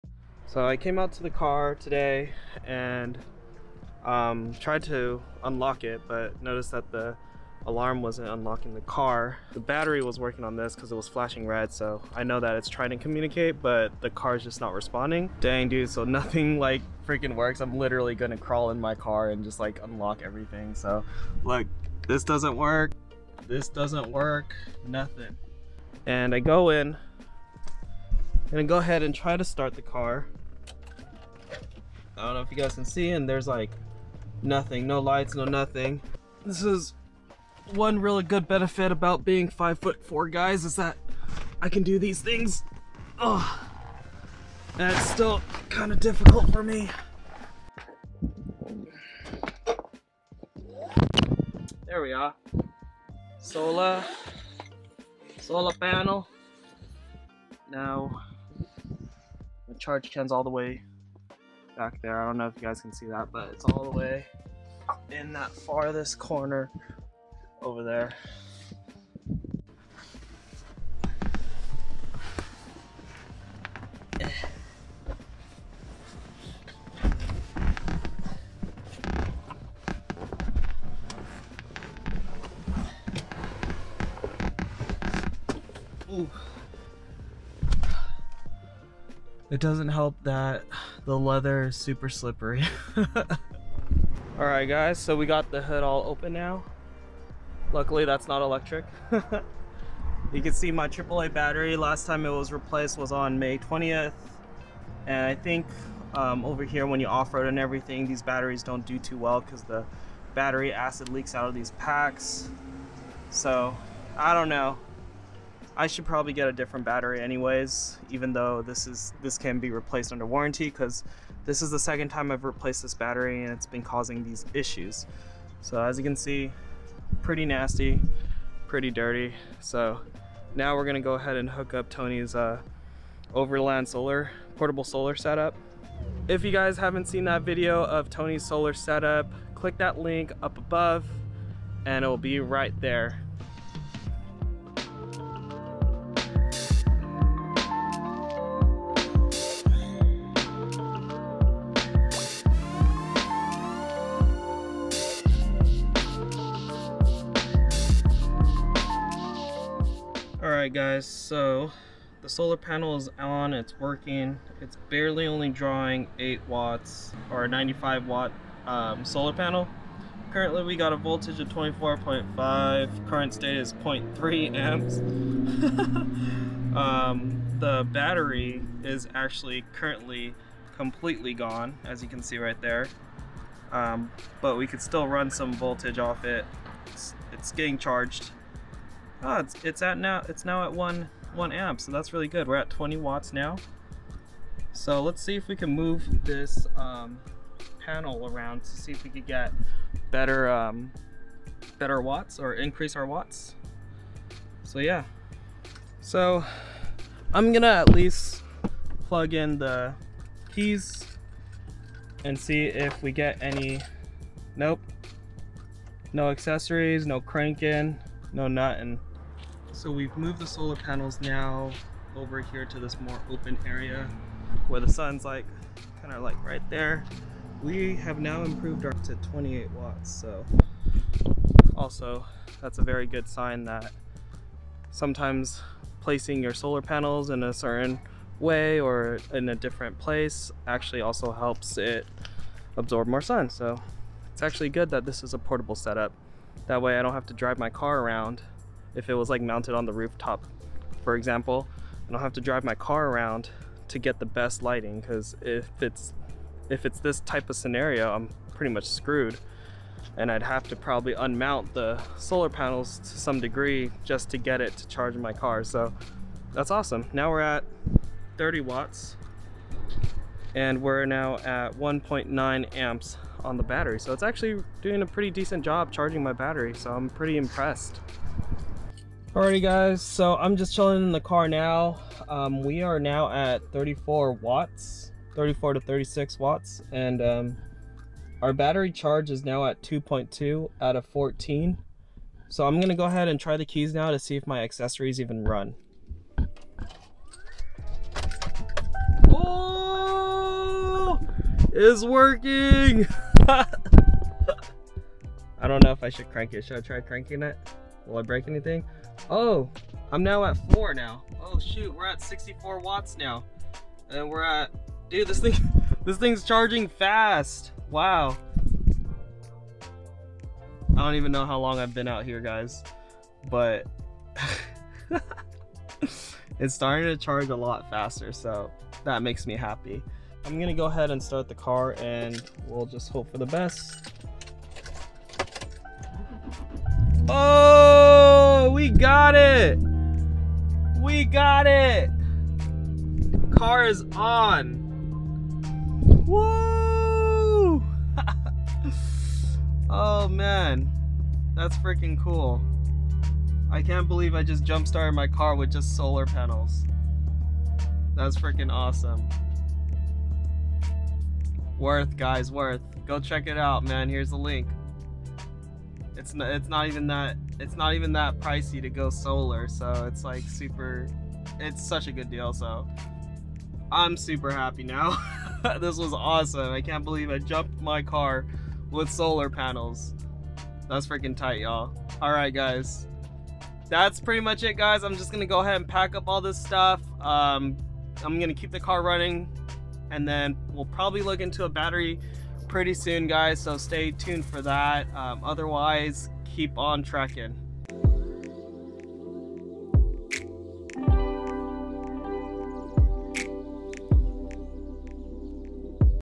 Is It Possible To SOLAR JUMP your Car?
I guess its time to change our battery. Our AAA has been failing us and hopefully we do not have a parasitic leak. But either way, glad we had our badluckoverland charge can setup. Although we did have jumping cables in the backseat, I was interested to see if the panels would be able to jumpstart the car, just because we can. Happy to say we are amazed and happy with the results.